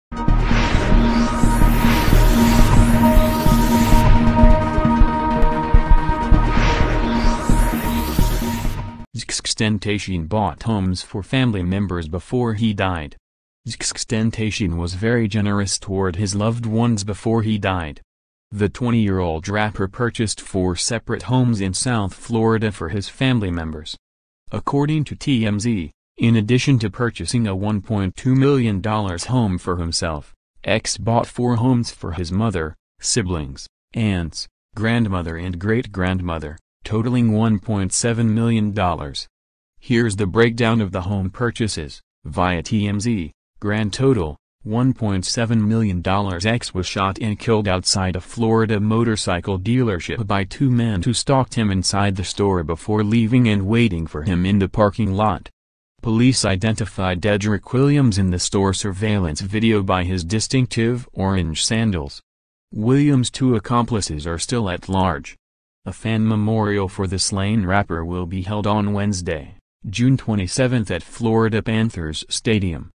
XXXTentacion bought homes for family members before he d ied
XXXTentacion bought homes for family members before he died.

XXXTentacion was very generous toward his loved ones before he died. The 20-year-old rapper purchased 4 separate homes in South Florida for his family members.

According to TMZ, in addition to purchasing a $1.2 million home for himself, X bought four homes for his mother, siblings, aunts, grandmother and great grandmother, totaling $1.7 million.

Here’s the breakdown of the home purchases (via